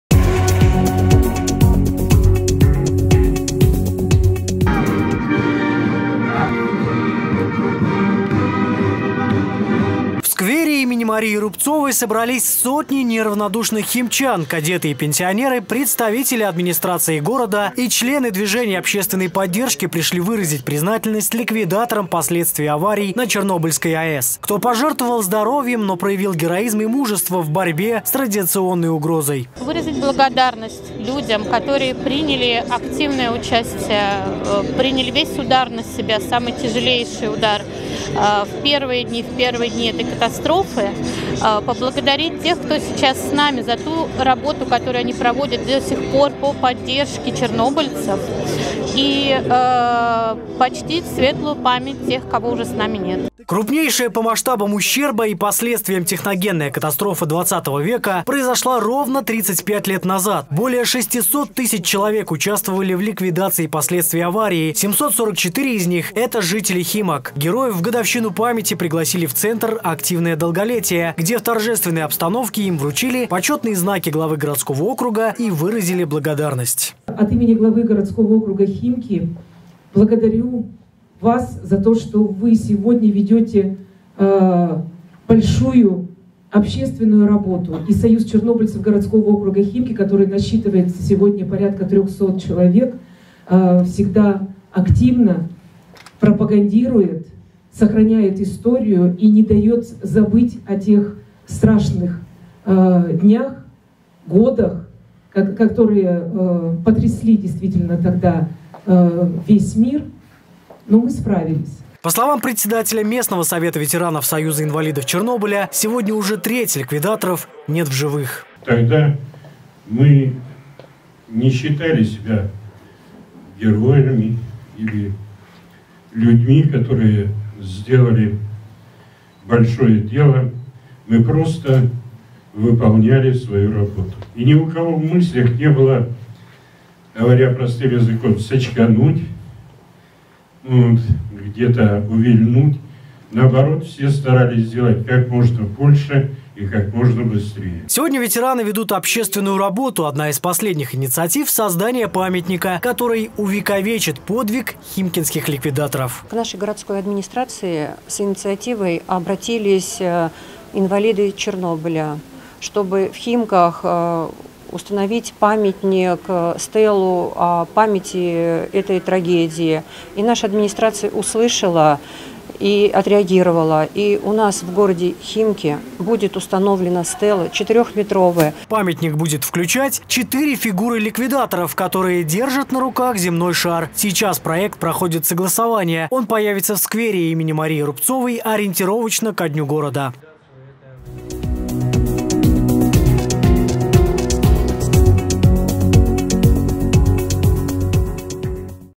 Химкинских ликвидаторов последствий аварии на ЧАЭС поблагодарили за подвиг. 26.04.2021
В сквере им. Марии Рубцовой собрались сотни неравнодушных химчан. Кадеты и пенсионеры, представители администрации города и члены Движения общественной поддержки пришли выразить признательность ликвидаторам последствий аварии на Чернобыльской АЭС. Кто пожертвовал здоровьем, но проявил героизм и мужество в борьбе с радиационной угрозой.